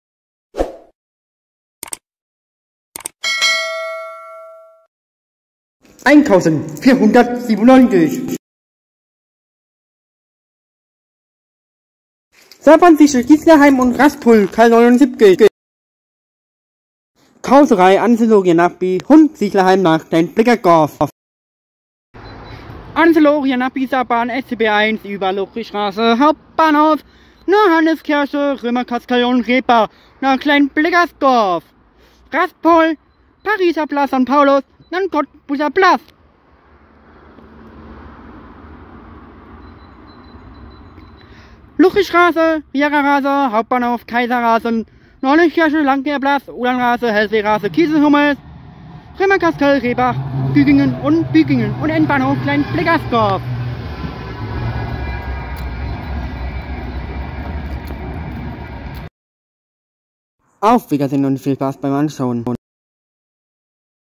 Saarbahn zwischen Siedlerheim und Rastpfuhl Teil 79 | @Trainspotter.Manuel.2025 | Job🎫 | Vlog 1497
Saarbahn zwischen Siedlerheim  £ Rastpfuhl  #79
Viedeonummer (1️⃣4️⃣9️⃣7️⃣)

Viedeoaufnehmen:  April. 2023

Mit dem Jobtickets für Saarbrücken

📍Saarbrücken-Malstatt
🚏Siedlerheim

1. 🚋 Kleinblittersdorf (1003 |  Angelo Via Nappli)

Wagen: 1
Fahezug:  1️⃣0️⃣0️⃣3️⃣
Linie: 87229
Ziel: Kleinblittersdorf
Über: Ludwigstraße , Hauptbahnhof

1️⃣0️⃣1️⃣3️⃣: Angelo Via Nappli

Fahrzeugnummer: 1013:  Angelo Via Nappli)

1. Fahrzeug
Saarbahn
🚋 STB1
Vom Siedlerheim
Über   Ludwigstraße - Hauptbahnhof  - Johenneskirche -   Römerkastell - Brebach
Nach  Kleinblittersdorf
🚏 Rastpfuhl - Pariser Platz/St. Paulus
🚋 Siedlerheim 》 Kleinblittersdorf
1️⃣0️⃣0️⃣3️⃣: Angelo Via Nappli

🚫:Keine Fahrplan Halt
🅰️:Abfahrt
📍:Standort
⬇️:Haltestelle
🔽:Über
🅱️:Ziel

1. Fahrzug

🚫Lebach-Jabach (Keine Fahrplan Halt)
🚫Lebach (Keine Fahrplan Halt)
🚫Lebach Süd (Keine Halt)
🚫Landsweiler Nord (Keine Fahrplan Halt)
🚫Landsweiler Süd (Keine Halt)
🚫Eiweiler Nord (Keine Fahrplan Halt)
🚫Eiweiler (Keine Fahrplan Halt)
🚫Heusweiler Kirchhof (Keine Halt)
🚫In der Hommersbach (Keine Halt)
🚫Heusweiler Markt (Keine Halt)
🚫Heusweiler Schulzentrum (Keine Halt)
🚫Walpershofen Mühlenstraße (Keine Halt)
🚫Walpershofen Mitte (Keine Halt)
🚫Walpershofen-Etzenhofen (Keine Halt)
🚫Gisorsstraße (Keine Fahrplan Halt)
🚫Güchenbach (Keine Fahrplan Halt)
🚫Riegelsberghalle (Keine Fahrplan Halt)
🚫Riegelsberg Kirchstraße (Keine Halt)
🚫Riegelsberg Rathaus (Keine Halt)
🚫Wolfskaulstraße (Keine Fahrplan Halt)
🚫Riegelsberg Süd (Keine Halt)
🚫Heinrichshaus (Keine Fahrplan Halt)
📍🅰️Siedlerheim 16:15
⬇️Rastpfuhl 16:16
⬇️Pariser Platz/ St. Paulus 16:18
⬇️Cottbuser Platz 16:19
🔽Ludwigstraße 1️⃣6️⃣:2️⃣1️⃣
⬇️Trierer Straße 16:23
🔽Hauptbahnhof 1️⃣6️⃣:2️⃣5️⃣
⬇️Kaiserstraße 16:26
🔽Johanneskirche 1️⃣6️⃣:2️⃣8️⃣
⬇️Landwerplatz 16:29
⬇️Uhlandstraße 16:31
⬇️Hellwigstraße 16:32
⬇️Kieselhumes  ️16:34
🔽Römerkastell 1️⃣6️⃣:3️⃣5️⃣
🔽Brebach 1️⃣6️⃣:3️⃣9️⃣
⬇️Güdingen 16:41
⬇️Bübingen 16:43
🅱️Kleinblittersdorf 16:46
🚫Ausermacher (Keine Fahrplan Halt)
🚫Hanweiler (Keine Fahrplan Halt)
🚫Sarrguemines (Keine Fahrplan Halt)